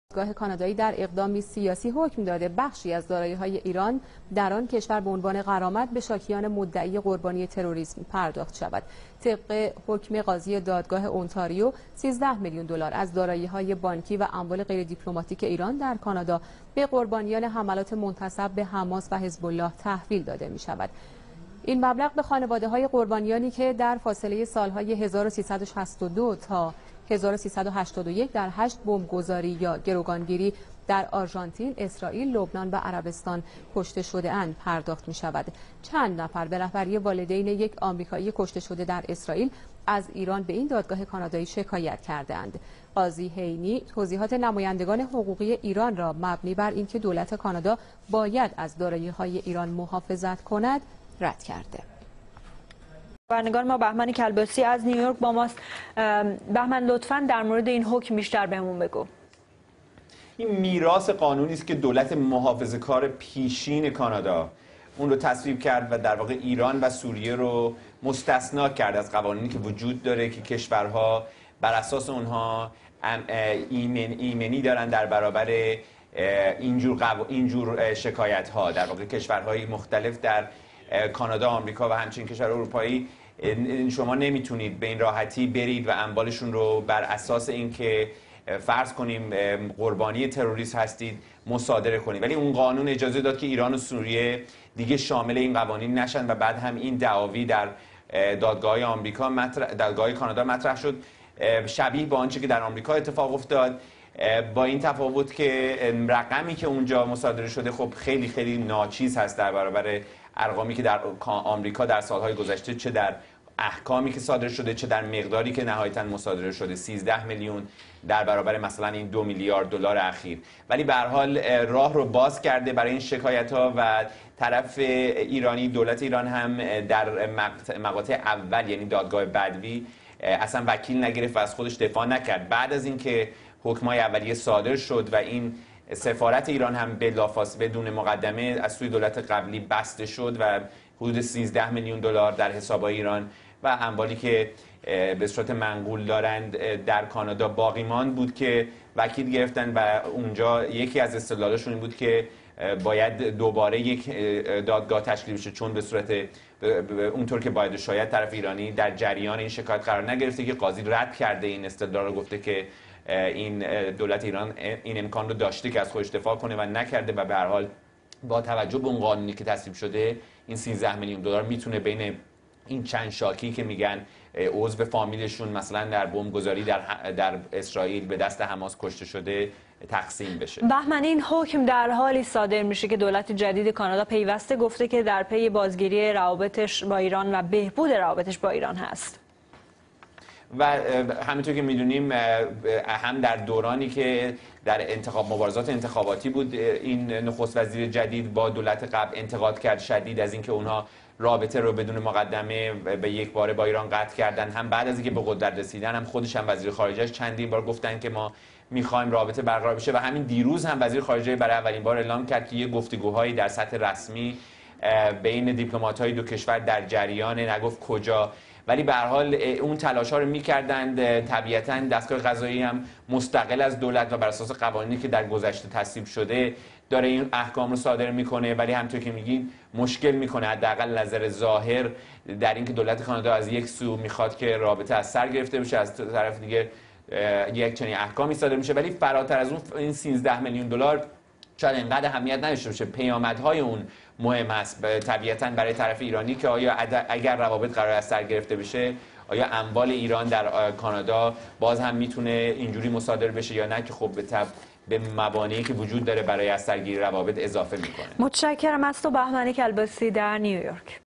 Confiscation of Iranian wealth in Canada and BBC Persian TV analyze
BBC Persian TV analyze confiscation of 13 Million USD by Canada on Friday 11 June